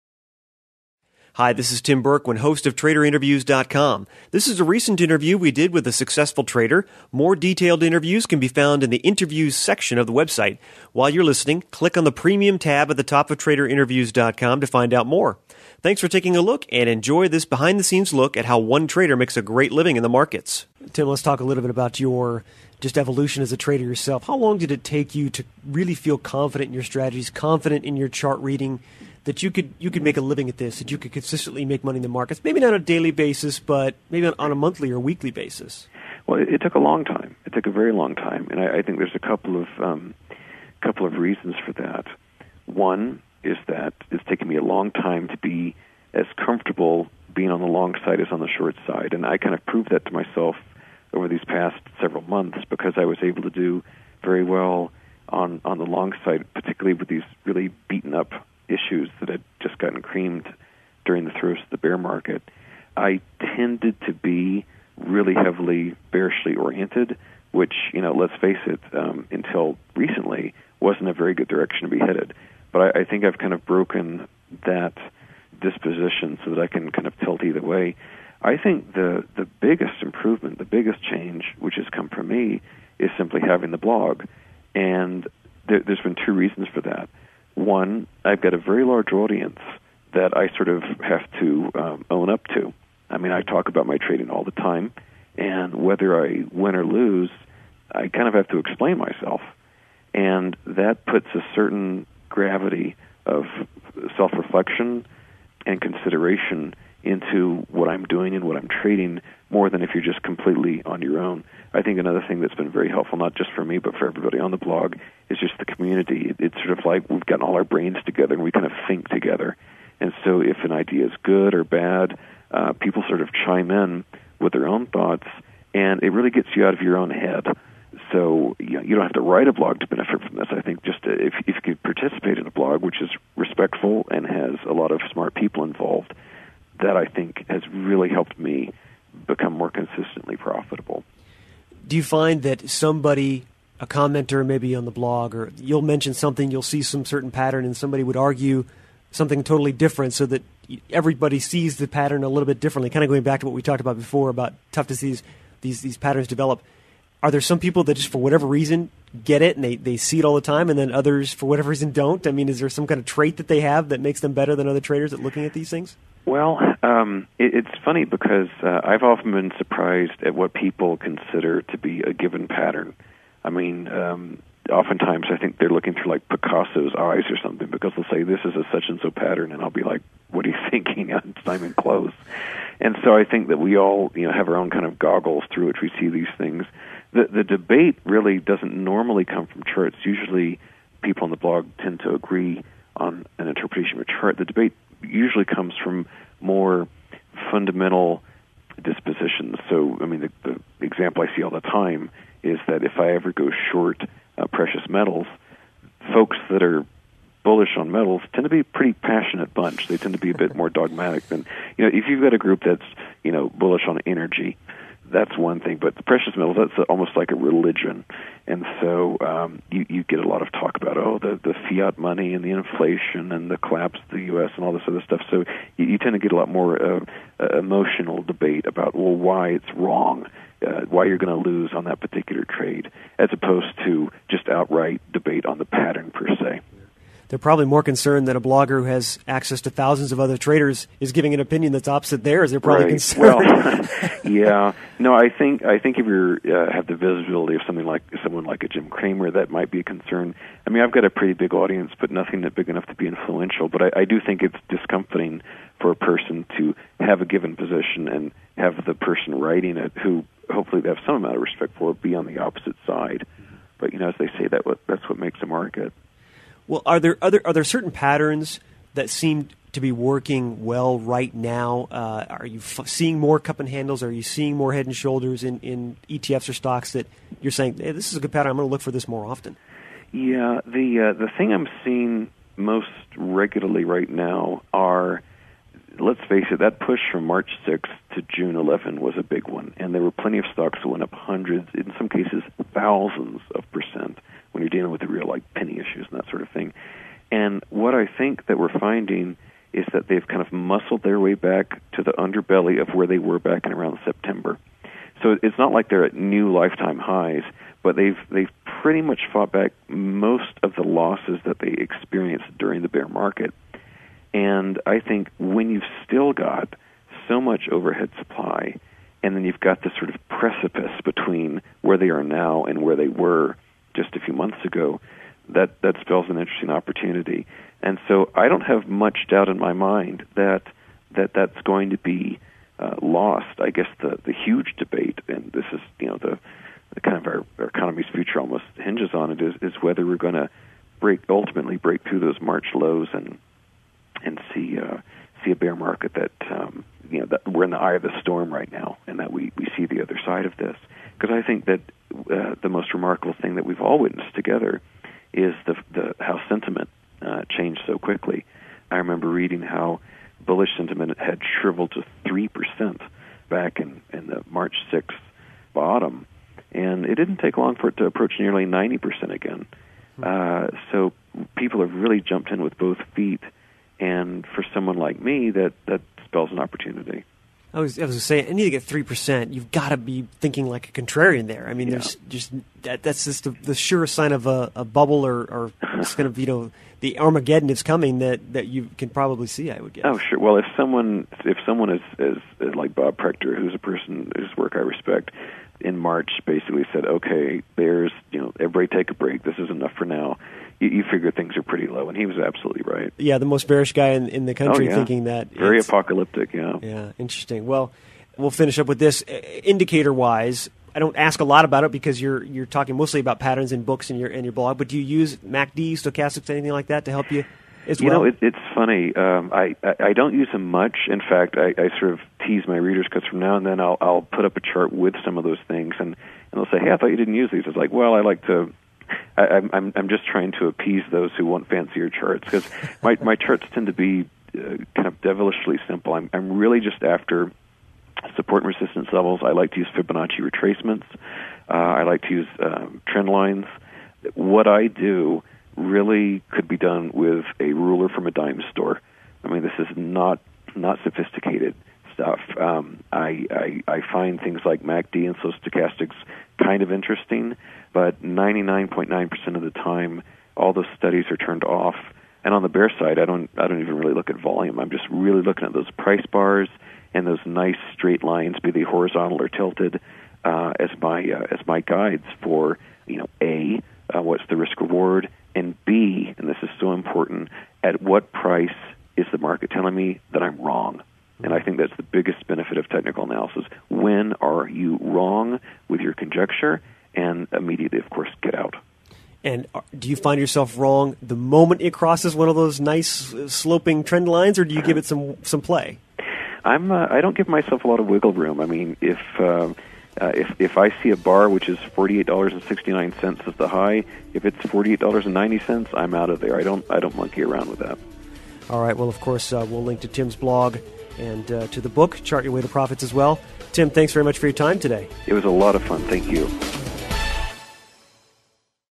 Part 3: Online Trading Strategies with Tim Knight
Professional trader Tim Knight talks about how individual traders can see and find different chart setups and patterns in the markets when they are stock trading online.  Tim then discusses where he thinks we are in the market and his stock market predictions for the future.